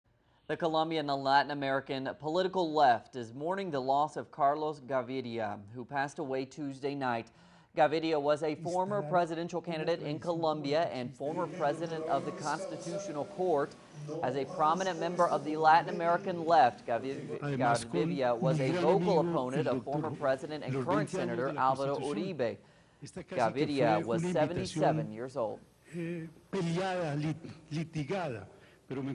Carlos Gaviria, Colombian leftist leader, dead at 77
Carlos Gaviria, Colombian leftist leader, one time presidential candidate, and former president of the country's Constitutional Court, died Tuesday night at the age of 77. He was a bitter opponent of past president and current senator Alvaro Uribe. teleSUR